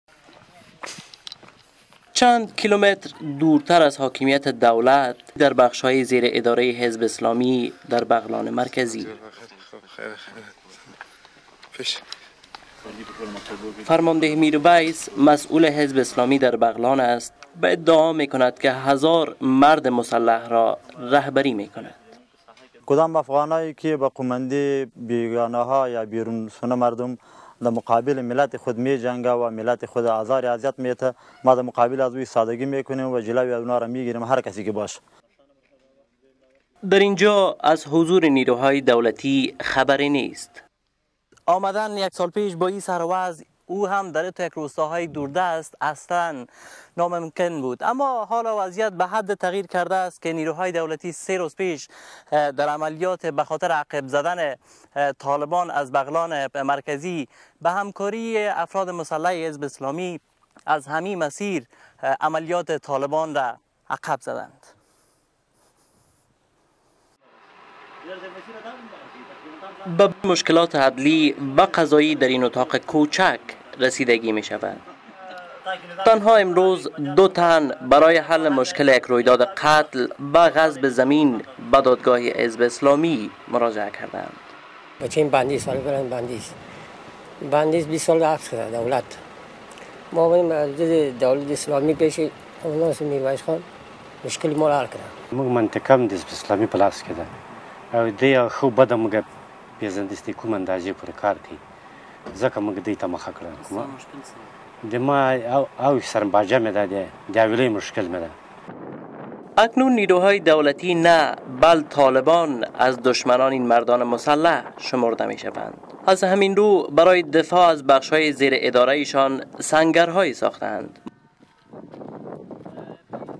Hizb-e-Islami Commander Ready To Support ANDSF Against Taliban
تفنگ‎داران حزب اسلامی در بغلان: با دشمنان مردم می‎جنگیم